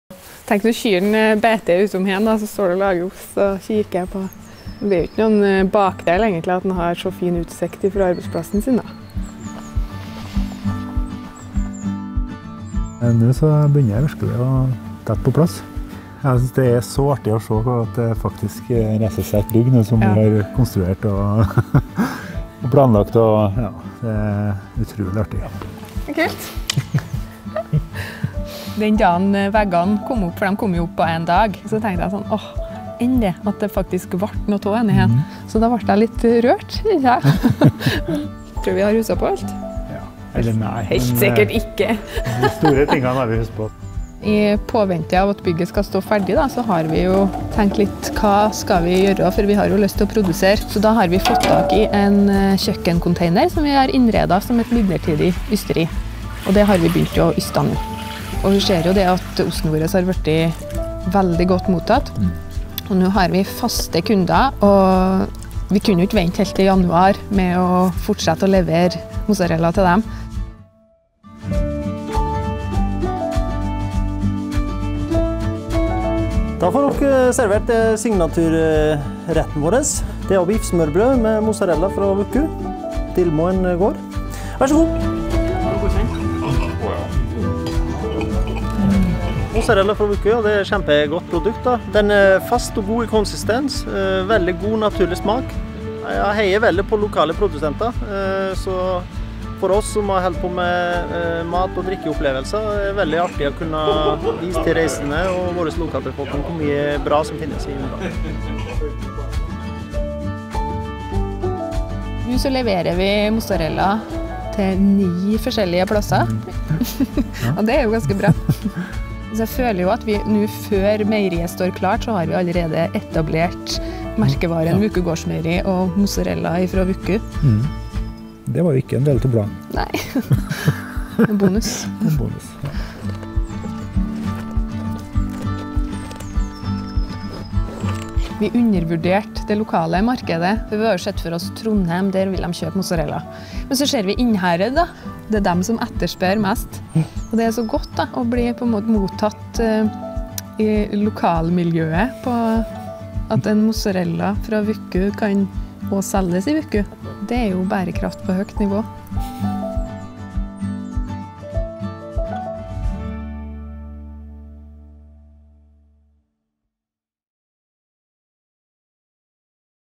Vuku Gårdsmeieri, episode 3: Drømmen som ble virkelig
Nå står et splitter nytt ysteri ute på gårdsplassen på Dillmoen Gård. Kathrine Lundgren og Arild Gaasvik kan åpne dørene til sin nye bedrift, etter drøyt et år med bratt læringskurve og mye arbeid. Nå skal det lages trøndersk, økologisk Mozzarella over en lav sko.

Les hele historien, fra starten på smn.no/gardsmeieri